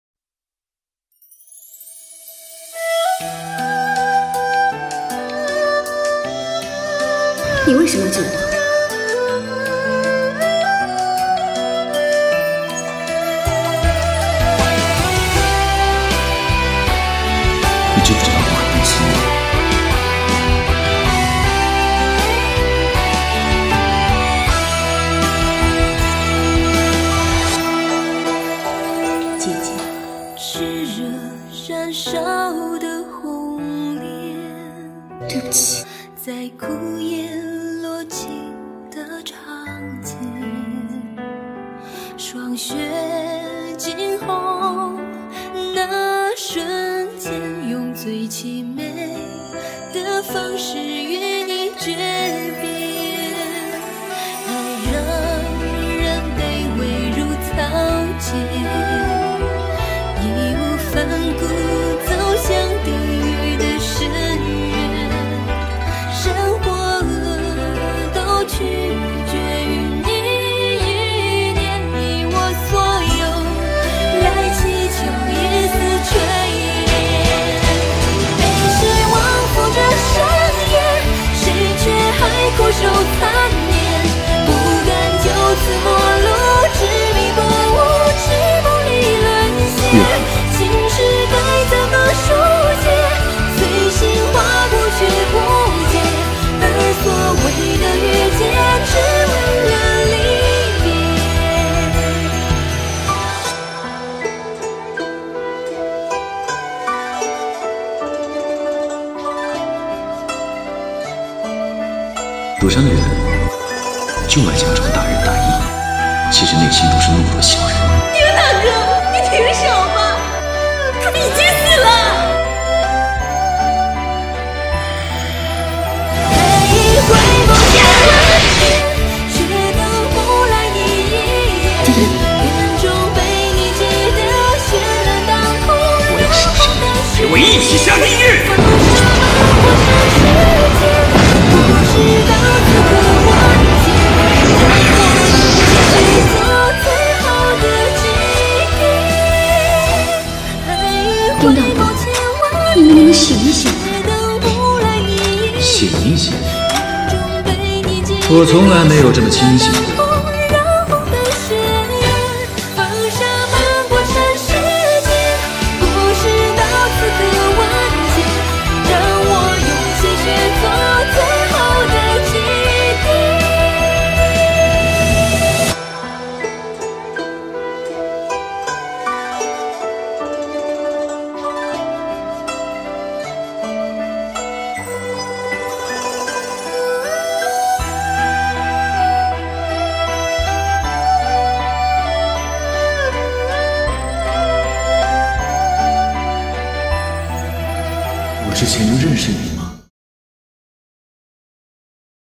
[FMV] Ding Yin & Yu'er《Part 2 of 5: From Two Different Worlds》(William Chan & Zhao Li Ying)
Part 2 of "From Two Different Worlds" (a fan-made mini drama).